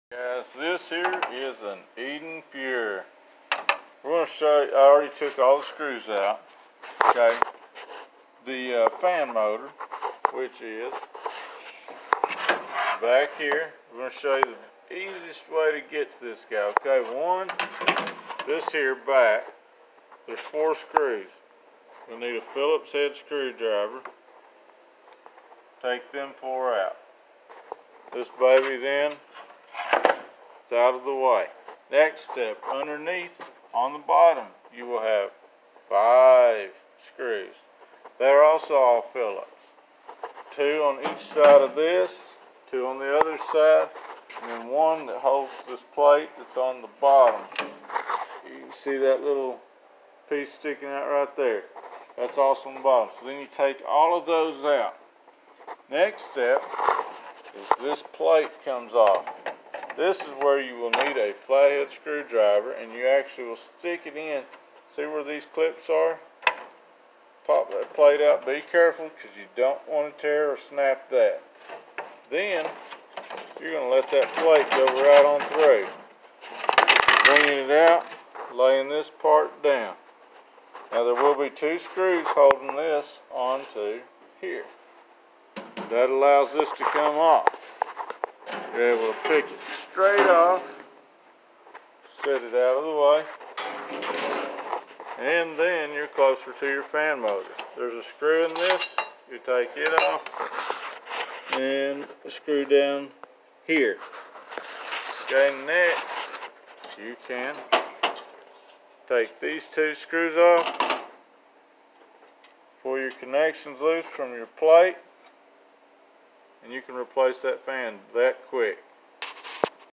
IN GOD - How to take apart and get to the fan blower on an EdenPURE Infrared Heater
Hope this was easy to under stand and if you need more help please ask I will be more than glad to see what I can do. I have a play list full of videos uploaded to look through. Thank you and May God Bless you